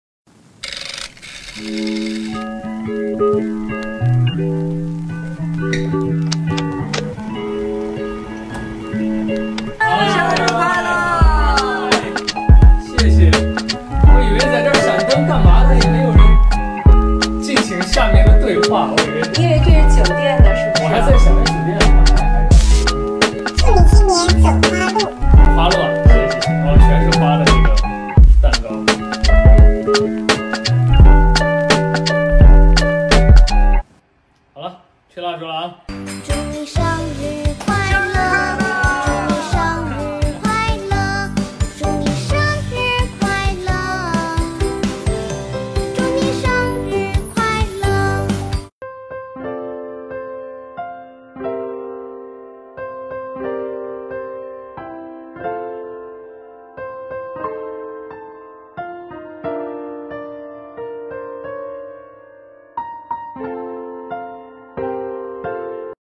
[Vietsub | Weibo PLV 11.4.2022] Vlog sinh nhật 33 tuổi
Nắm bắt cơ hội chuẩn bị một bất ngờ sinh nhật nhỏ, hy vọng boss @NhậmGiaLuânAllen một tuổi mới, tiếp tục đi trên đường hoa! Mãi luôn vui vẻ, mãi là thiếu niên!❤️
#️⃣Nhậm Gia Luân 411 sinh nhật vui vẻ
Cre: weibo @任嘉伦工作室
Vietsub by pun